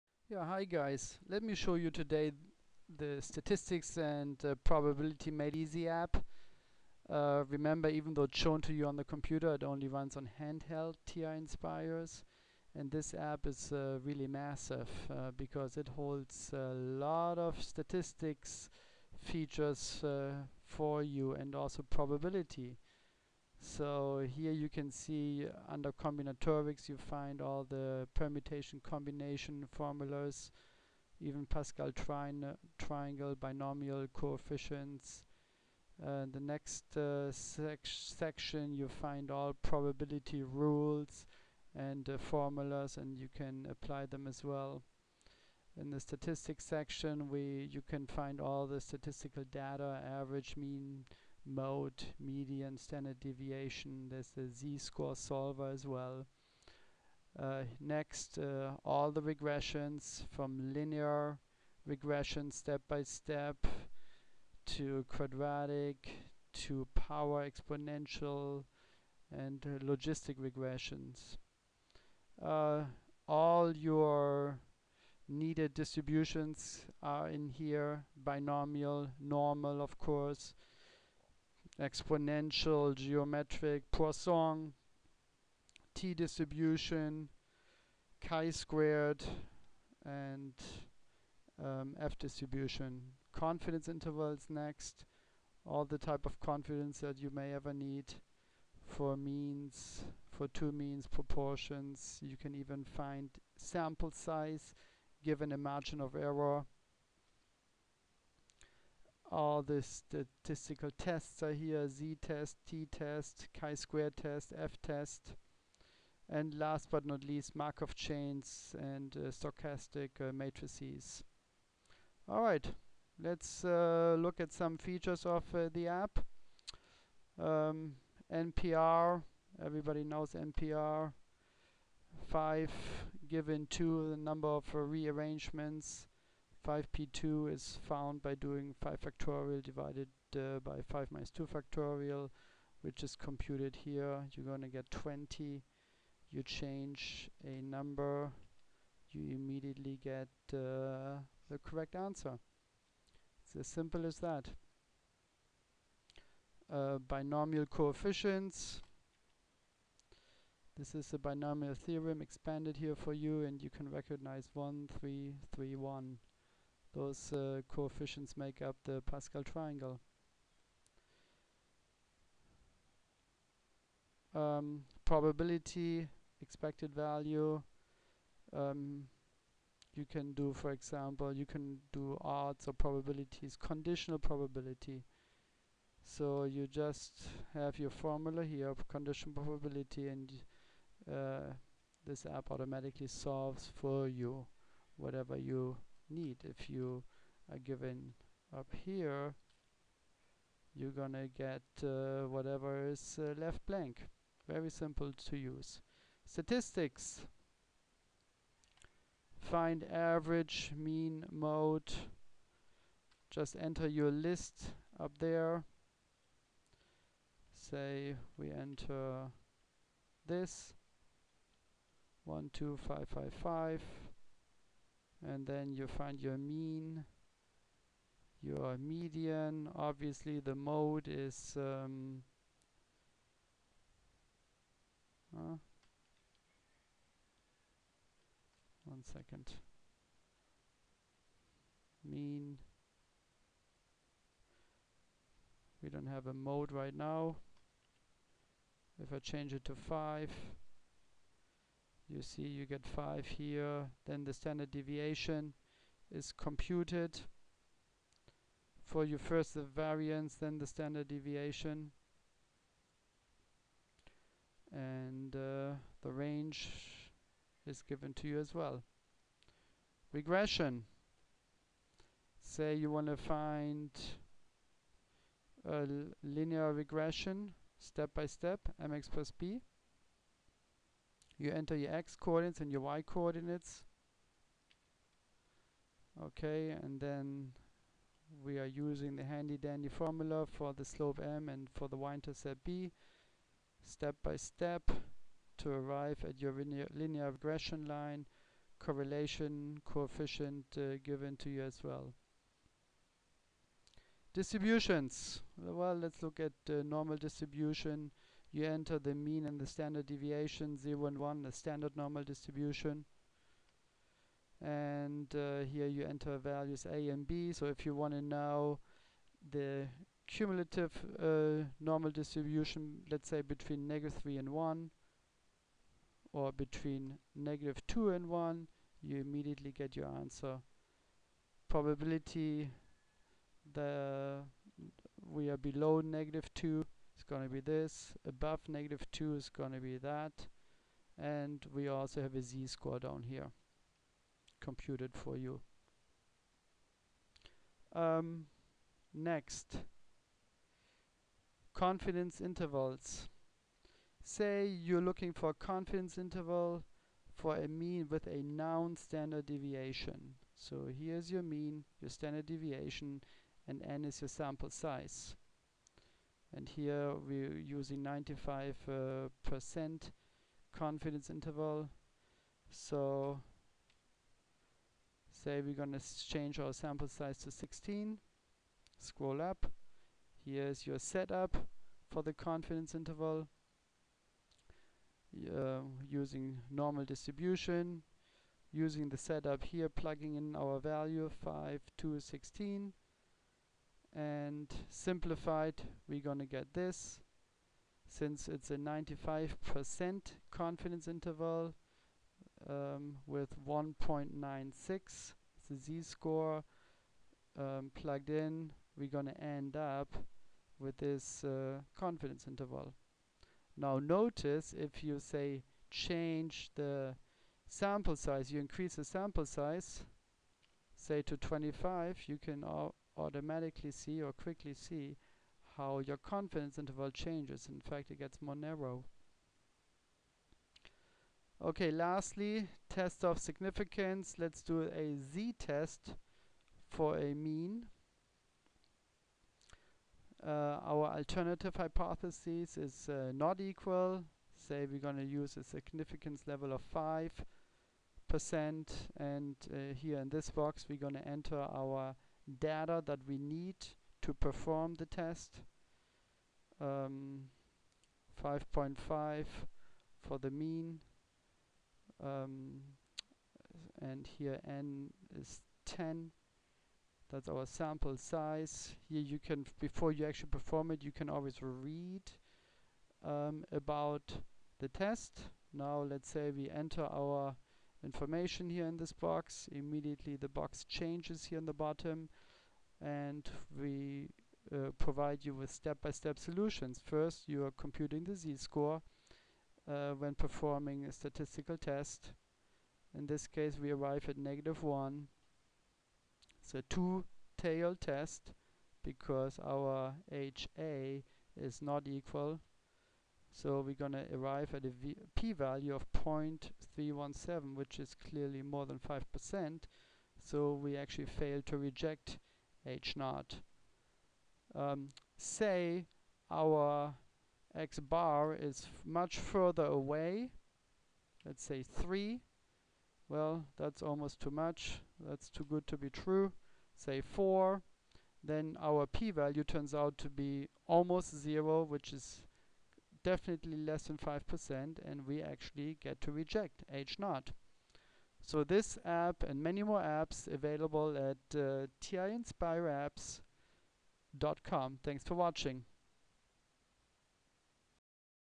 Tinspire CX: Statistics Solver - Step by Step
Step by Step statistics on the ti nspire cas cx :
Mean, Median, Std Dev, Normal and Binomial Distribution, Discrete and Continuous Distributions, Z-Solver, All types of Regression, Expected Values, 1-var and 2-var Statistics, Data Analysis, Random Numbers, Statistical Tests, Confidence Intervals, Non-Parametric Tests, Markov Chains,  Business Statistics, Decision Analysis, Moment Generating Functions, and much more.

This app covers 3 semesters of College Statistics and AP Statistics.

Free Trials available.